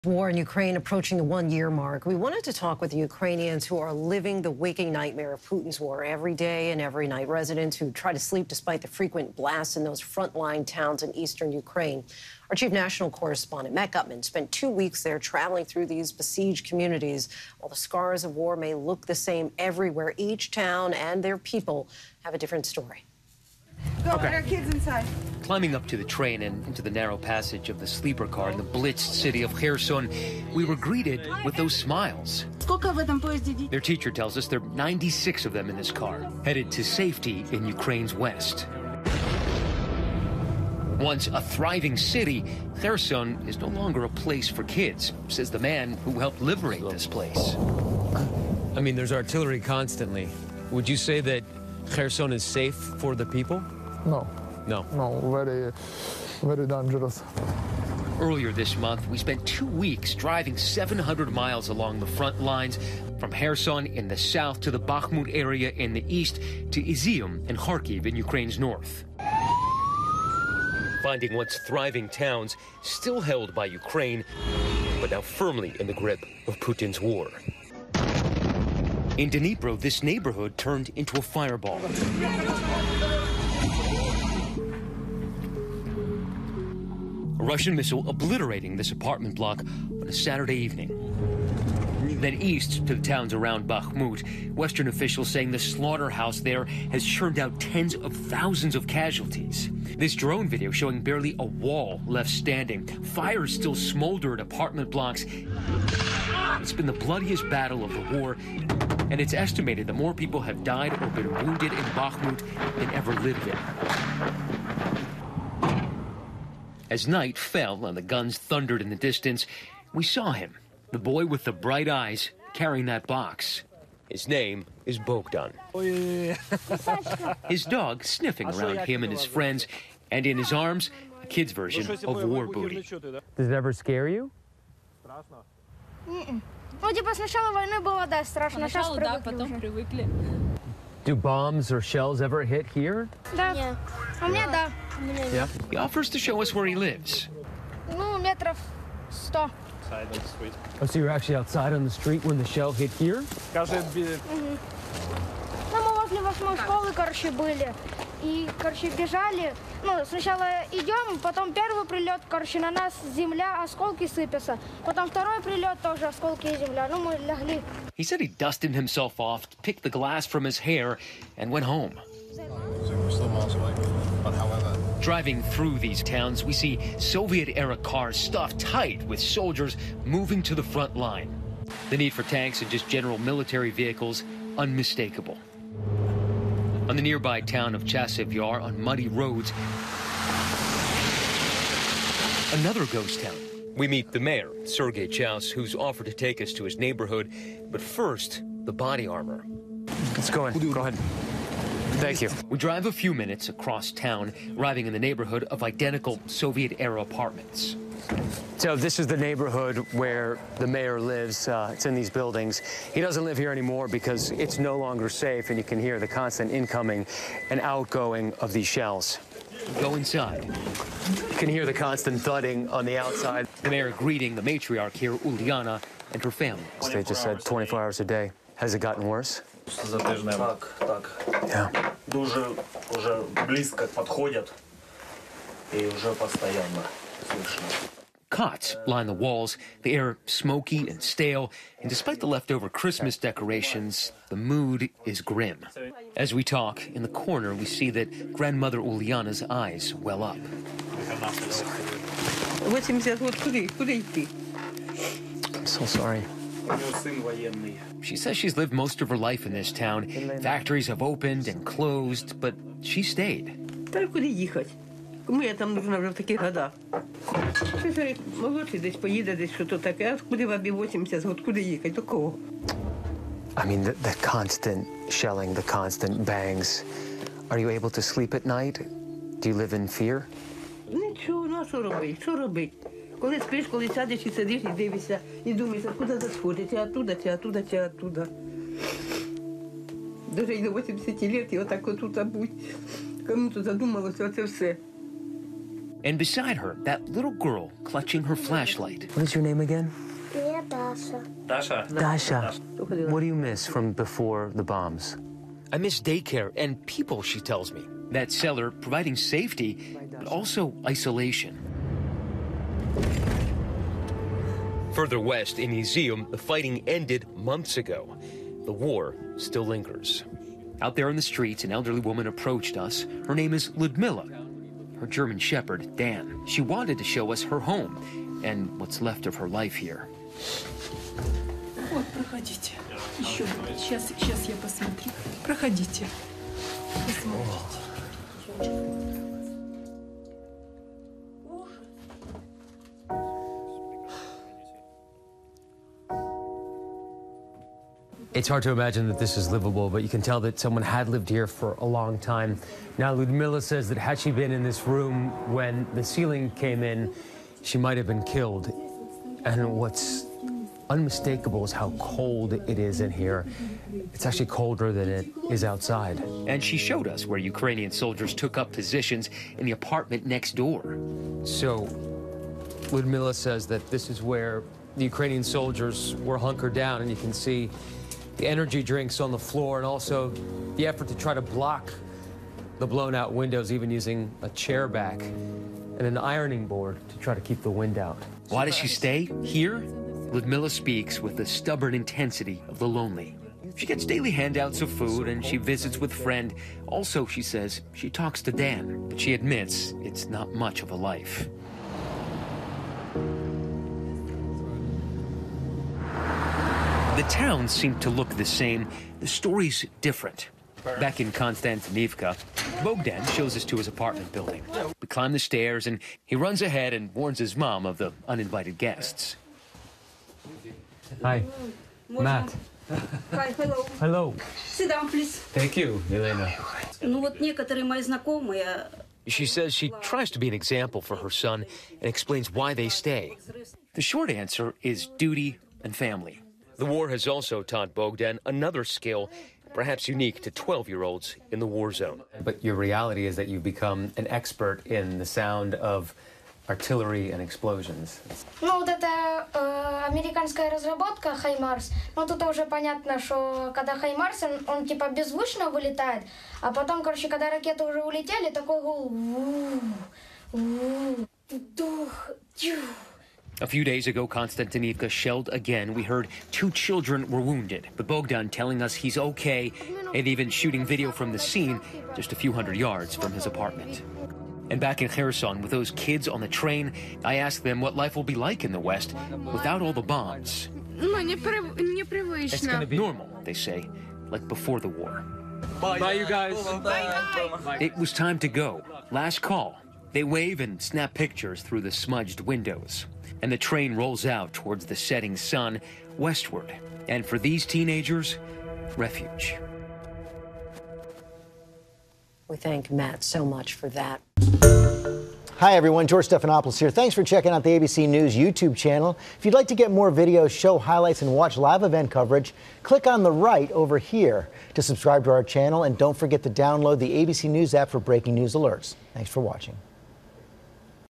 ABC News' Matt Gutman reports on the war scars in Ukraine's frontline towns: An inside look
ABC News' Matt Gutman reports on spending two weeks driving 700 miles along eastern Ukraine's front lines and has a moving look at the human toll of a yearlong war and the children left behind.